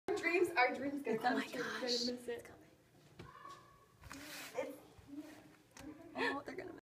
butterfly coming out of Chrysalis! pt 1
Caught this amazing thing happening by accident and what an accident it was!! God's creations are so AMAZING